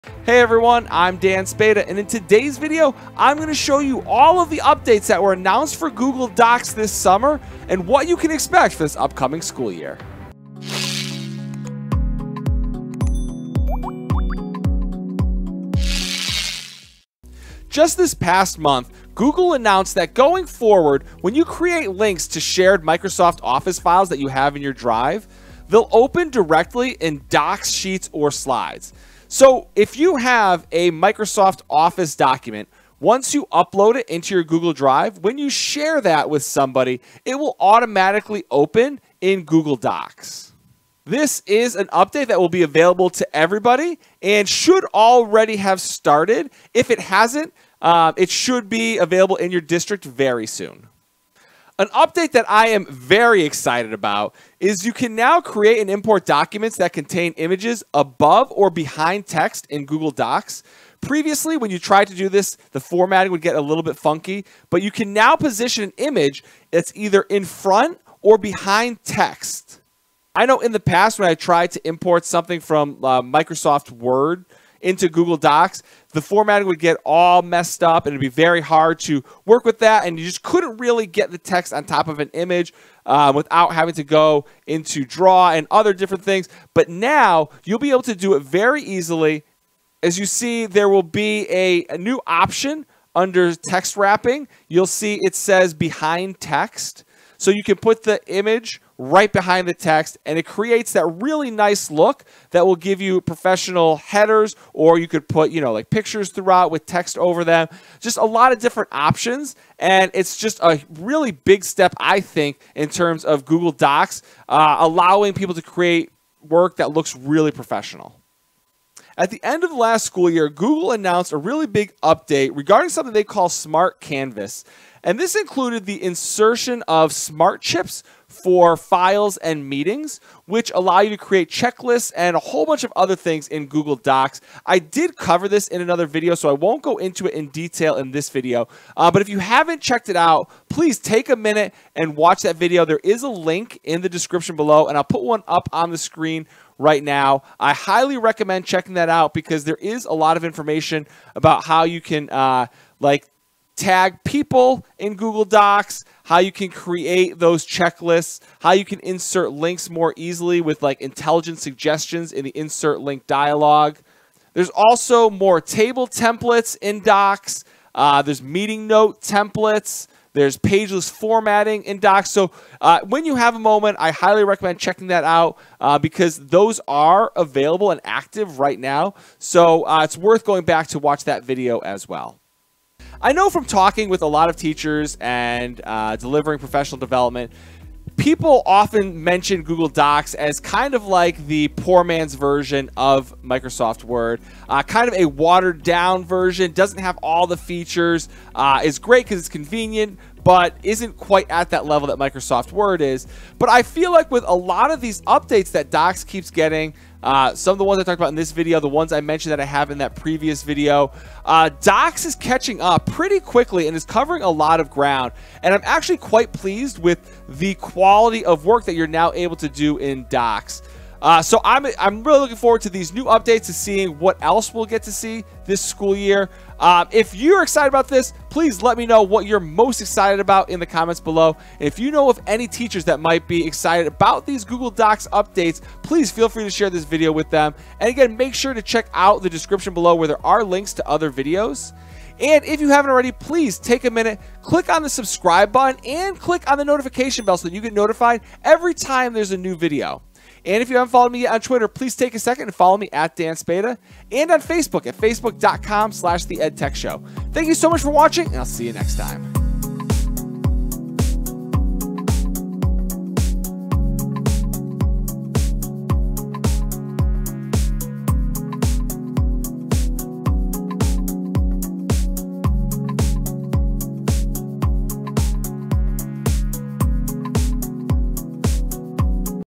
Google Docs Updates For The 2021-2022 School Year
GoogleDocs GoogleDocsTutorial GoogleDocsUpdate Google Docs Updates For The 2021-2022 School Year. This video takes a look at the Google Docs updates that were announced this summer, and everything you need to know for the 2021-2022 school year!

How To Add A Background Image To Google Meet Using The New Built-In Background Change Tool!